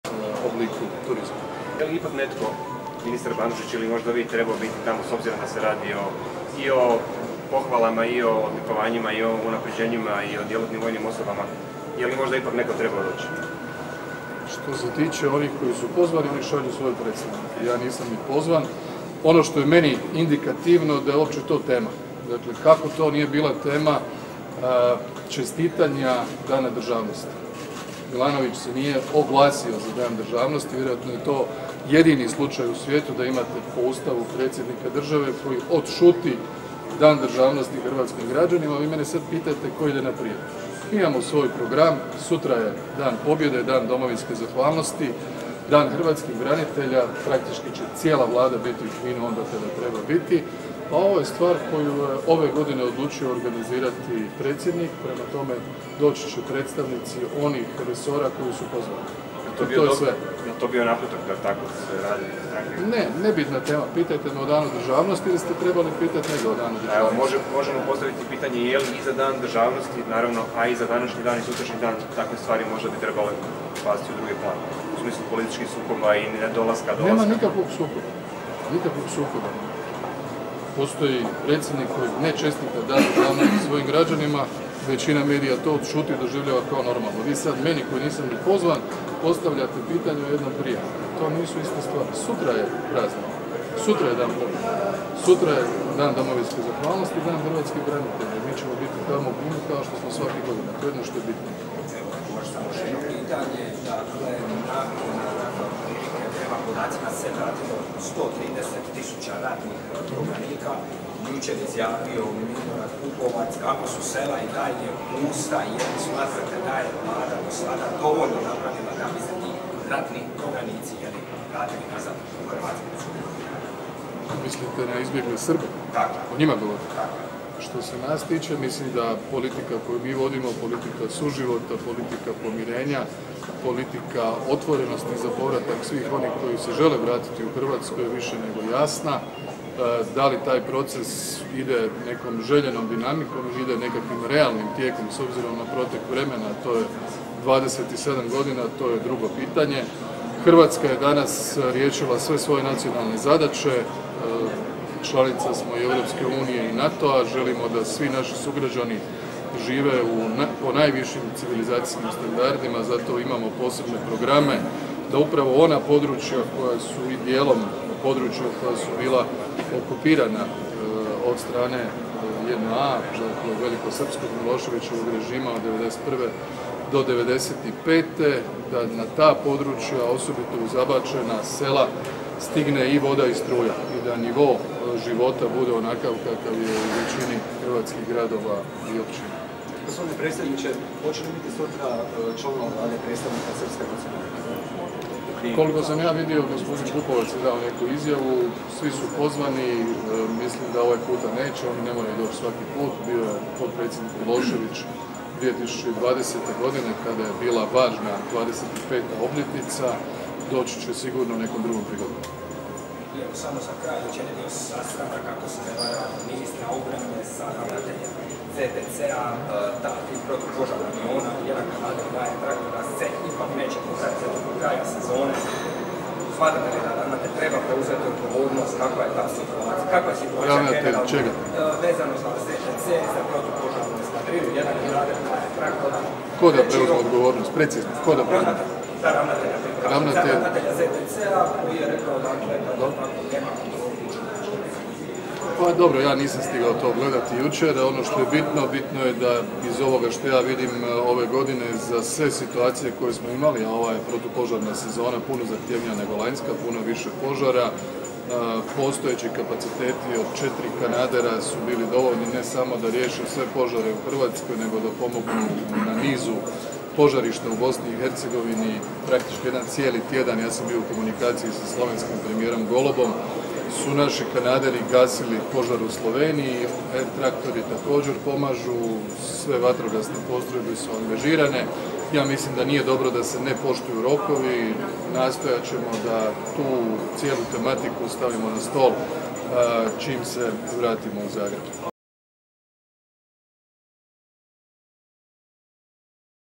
Plenković o Milanoviću: Nije se oglasio za Dan državnosti, to je jedini slučaj u sviijetu
Sastanak se s predstavnicima turističkog sektora, županima priobalnih županija i gospodarstvenicima u Petrčanima.